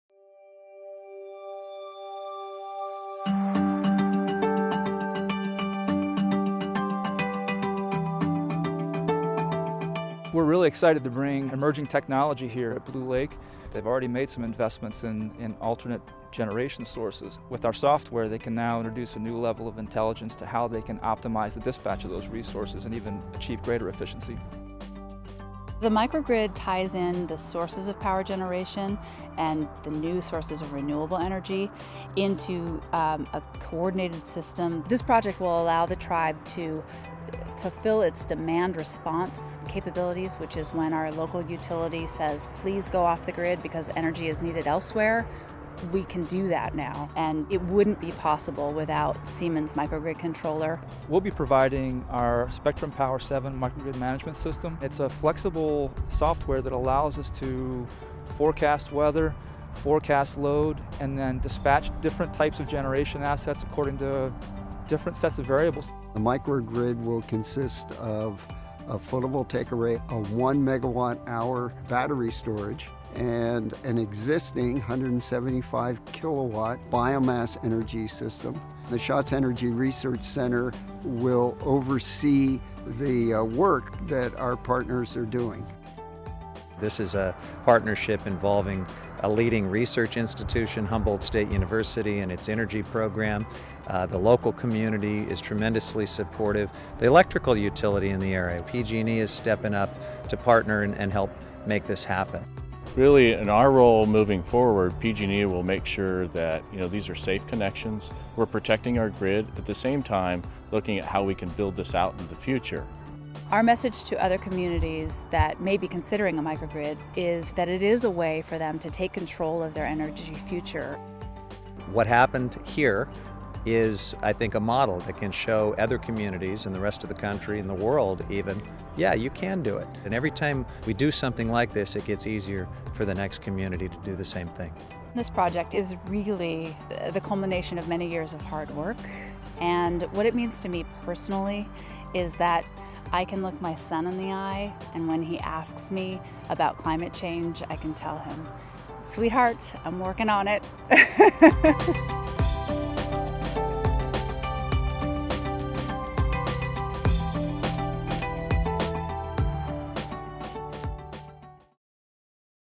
Blue Lake Rancheria Microgrid
Blue Lake Rancheria, a Native American reservation in Northern California, and Humboldt State University's Schatz Energy Research Center have partnered with Siemens to build a low-carbon community microgrid to power the government offices, economic enterprises, and critical Red Cross safety shelter-in-place facilities across 100 acres. The microgrid, funded in part through a $5 million grant from the California Energy Commission's Electric Program Investment Charge (EPIC) program, will be powered by a 0.5 MW solar photovoltaic installation, 950 kWh battery storage system, a biomass fuel cell system, and diesel generators. Operators will manage and control these energy resources with Siemens Spectrum Power Microgrid Management System (SP MGMS) software. The microgrid will allow the reservation to operate independently of the power grid in coordination with local utility Pacific Gas & Electric. This project constitutes the largest solar array in Humboldt County, California and is estimated to reduce 150 tons of carbon per year.